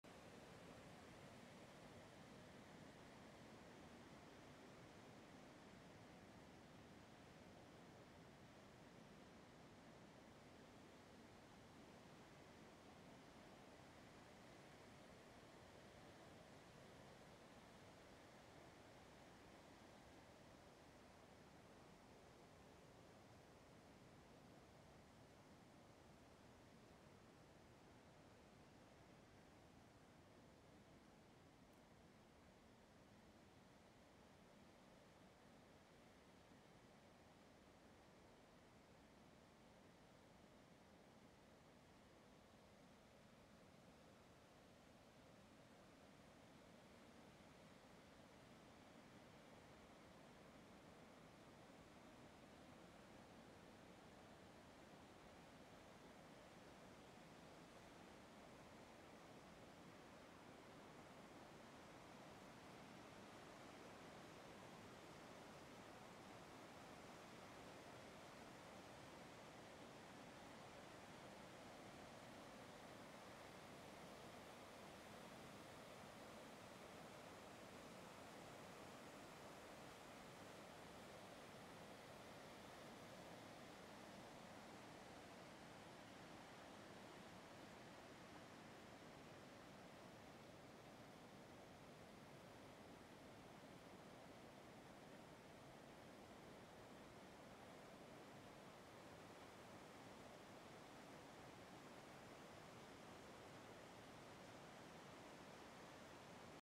SENDEIRO RIO UMIA CORTADO EN SEGADE 20-01-2013
O sendeiro tradicional que leva a fervenza de Segade pola beira do rio umia esta cortado polo peche da finca que rodea o muiño e pola desfeita dunha pasarela afectada polas riadas